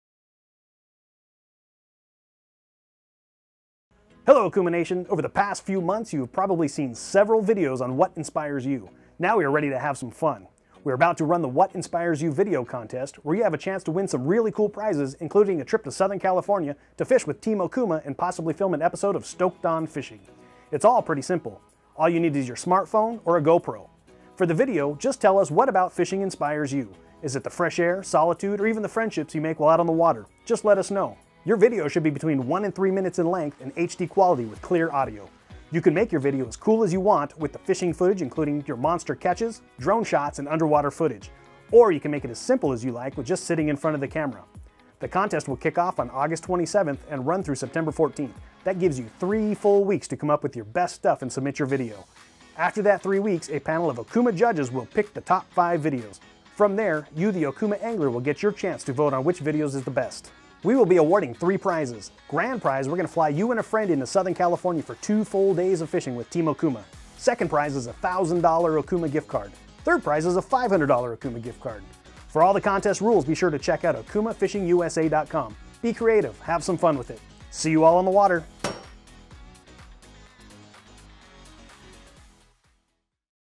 What Inspires You Video Contest Instructions
Dave Brown of Okuma Fishing Tackle gives a quick rundown on how to enter the What Inspires You Video Contest. Users must submit their 1-3 minute video between August 27th and September 22nd. For more details please visit: